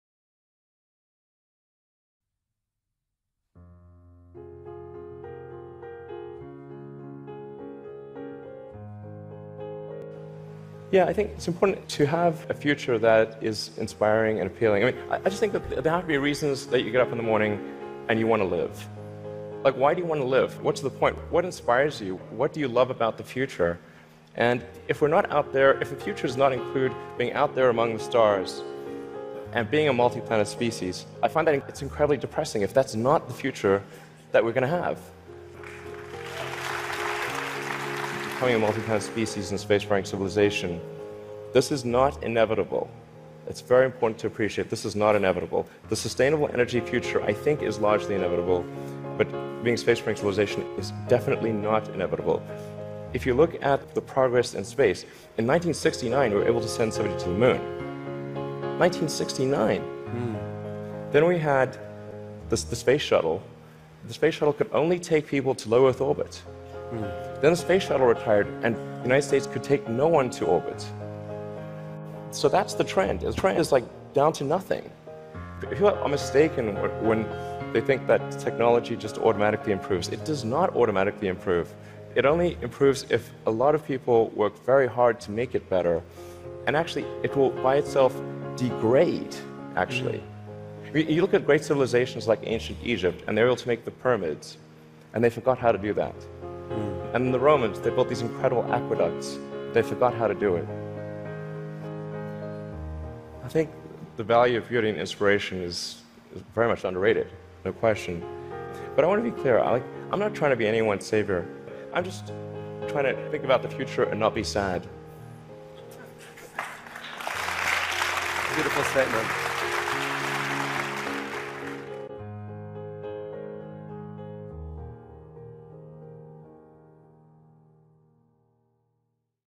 Best Inspirational Speech |Elon Musk's Most Passionate Speech
Taken from Elon Musk's interview on TED Vancouver 2017. This is a compilation of his most inspirational moments.

falls under creative commen